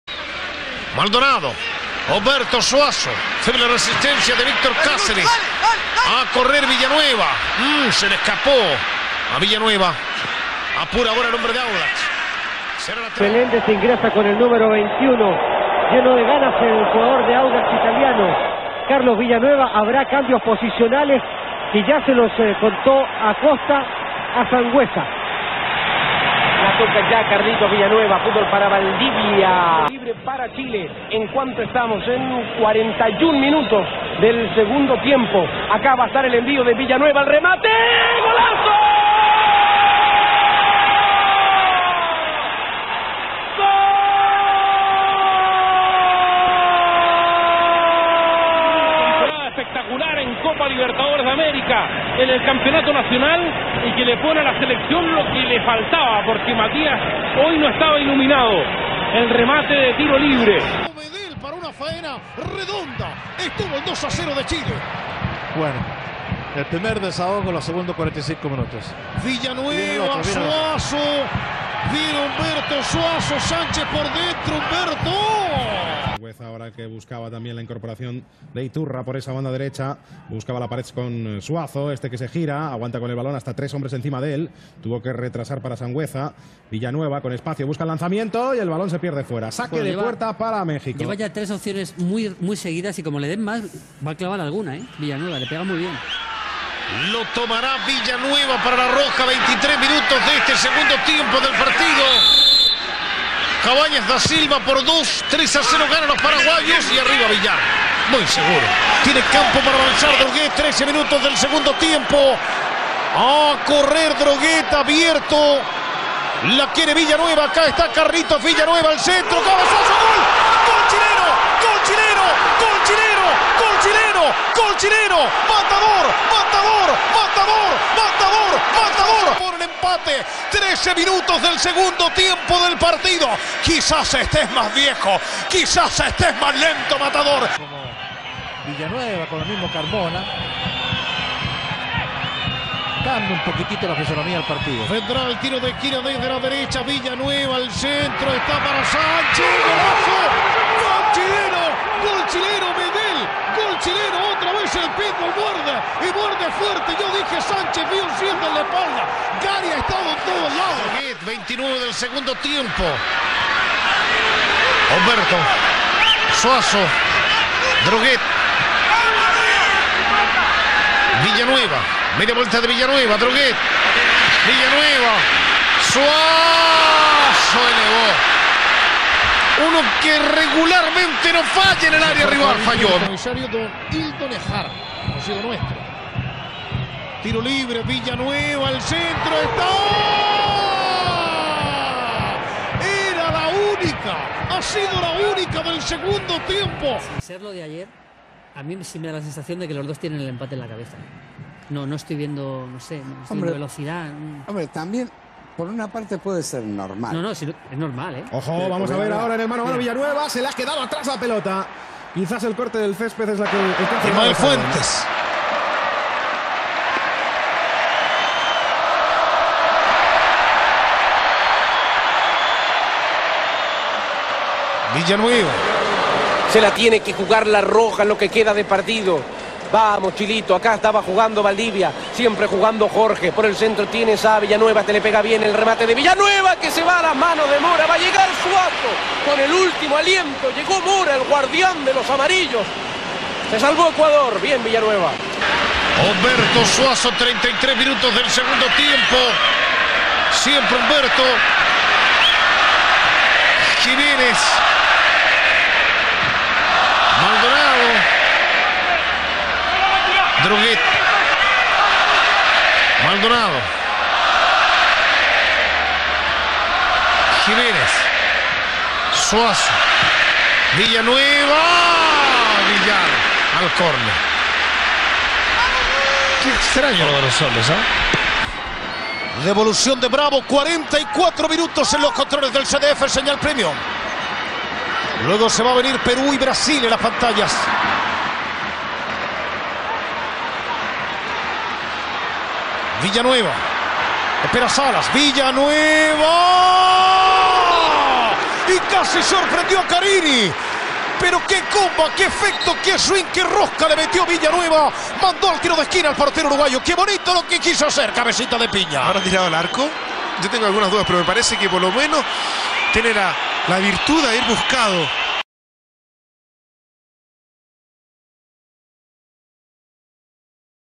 🔴 Carlos Villanueva, selección chilena
Un repaso de su carrera defendiendo a la selección chilena en partidos oficiales.